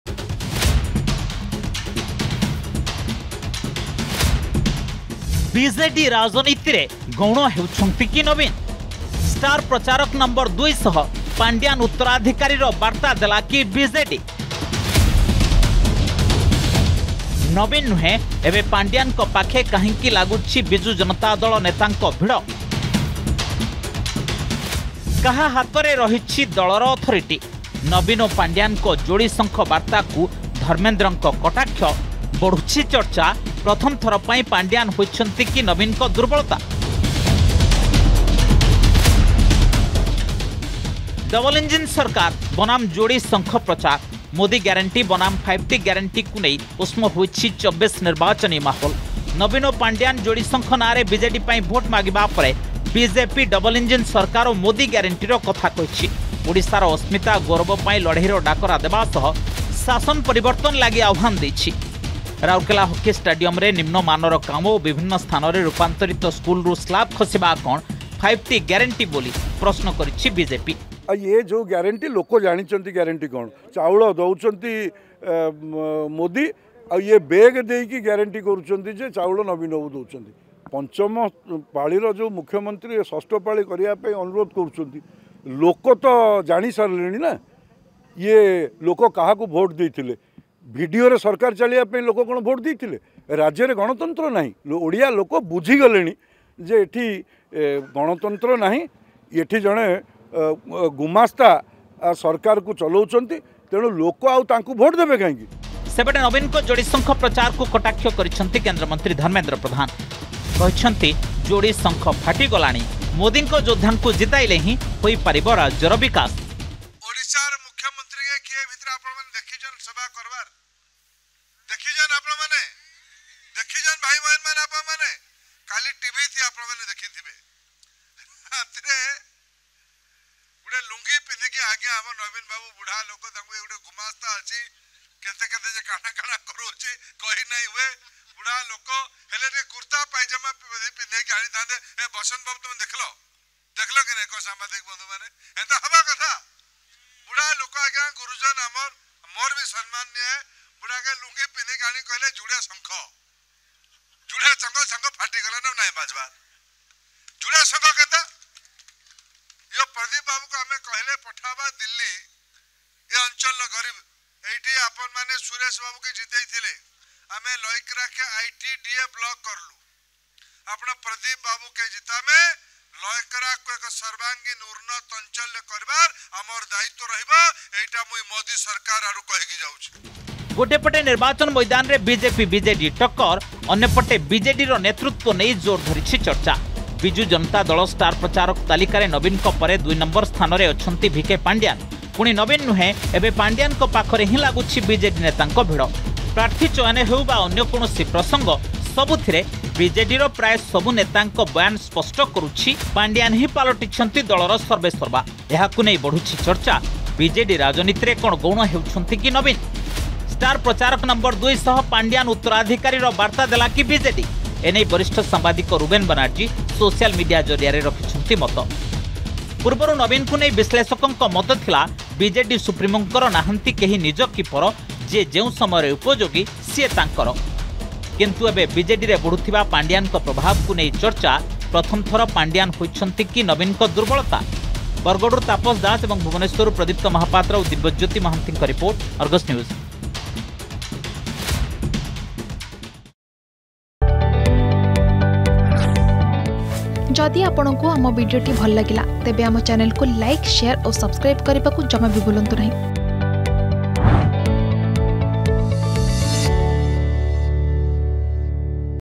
ବିଜେଡି ରାଜନୀତିରେ ଗୌଣ ହେଉଛନ୍ତି କି ନବୀନ ? || Election 2024 || Dharmendra Pradhan
ବିଜେଡି ରାଜନୀତିରେ ଗୌଣ ହେଉଛନ୍ତି କି ନବୀନ ? ଷ୍ଟାର୍ ପ୍ରଚାରକ ନଂ-୨ ସହ ପାଣ୍ଡିଆନ ଉତ୍ତରାଧିକାରୀର ବାର୍ତ୍ତା ଦେଲା କି ବିଜେଡି ? ନବୀନ ନୁହେଁ ଏବେ ପାଣ୍ଡିଆନଙ୍କ ପାଖେ କାହିଁକି ଲାଗୁଛି ବିଜେଡି ନେତାଙ୍କ ଭିଡ଼ ? କାହା ହାତରେ ରହିଛି ବିଜେଡି ଦଳର ଅଥରିଟି ? ନବୀନ ଓ ପାଣ୍ଡିଆନଙ୍କ ଯୋଡ଼ି ଶଙ୍ଖ ବାର୍ତାକୁ ଧର୍ମେନ୍ଦ୍ରଙ୍କ କଟାକ୍ଷ।